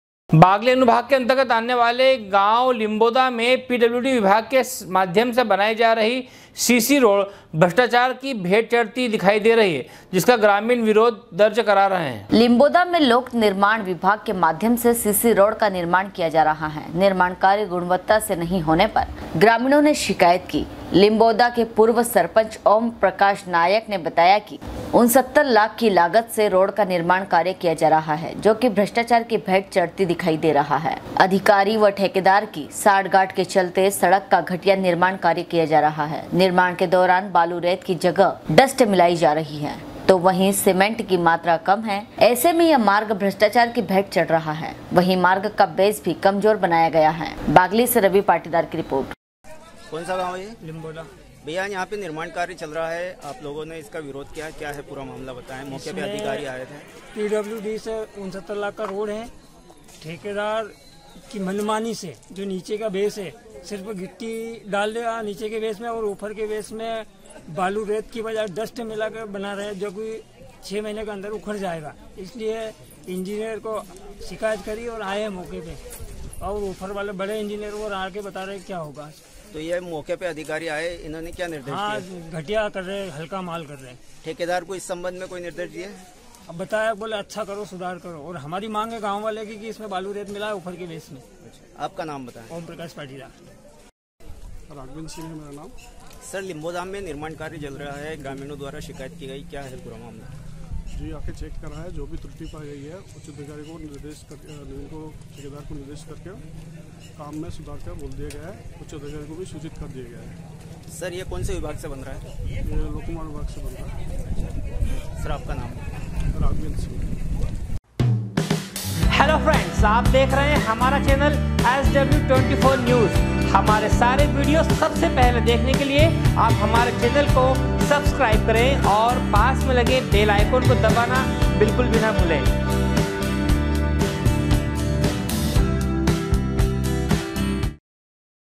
लिम्बोदा में चूक विभाग के माध्यम से बनाई जा रही सीसी रोड़ भ्रष्टाचार की भेंट चढ़ती दिखाई दे रही है,
बागली अनुविभाग के अंतर्गत आने वाले गांव लिम्बोदा में चूक विभाग के माध्यम से बनाई जा रही सीसी रोड़ भ्रष्टाचार की भेंट चढ़ती दिखाई दे रही है, जिसका ग्रामीण विरोध कर रहे हैं